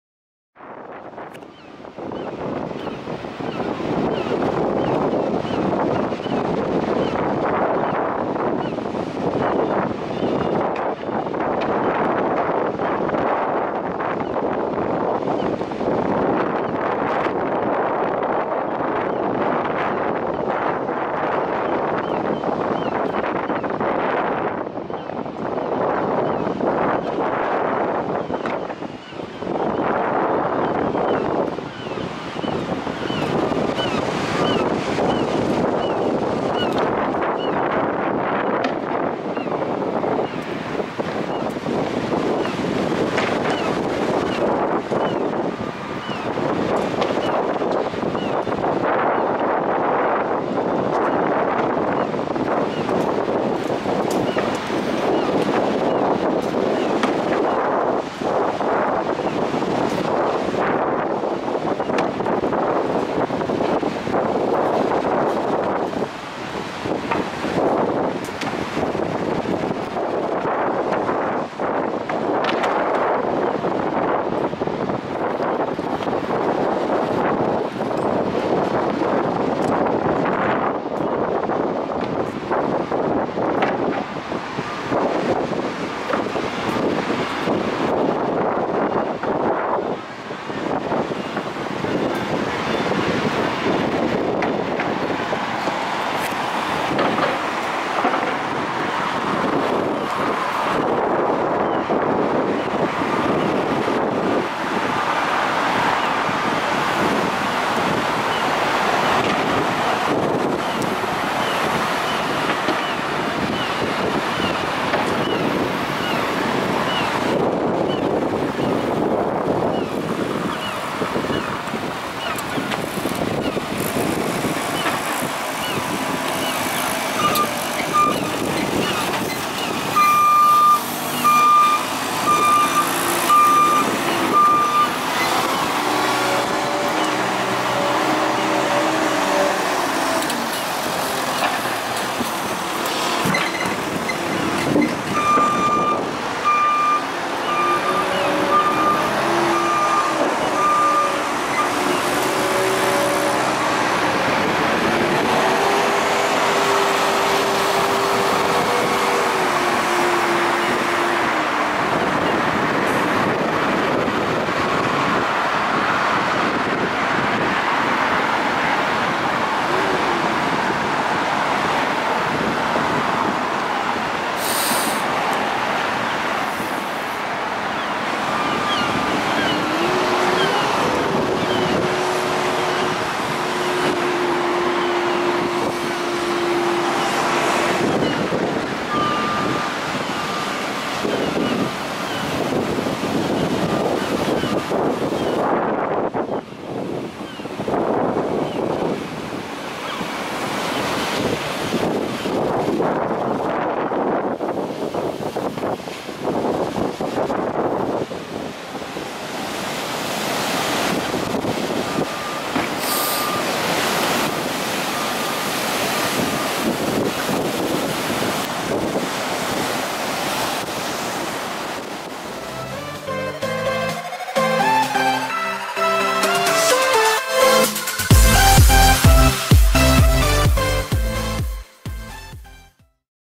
Rough Sea near North Pier and setting things up near Comedy Carpet
Also videos of areas surrounding Blackpool. going north we have Bispham, Cleveleys, maybe as far as Fleetwood and Knott End. In the other direction, going south we will be visiting St Annes and Lytham.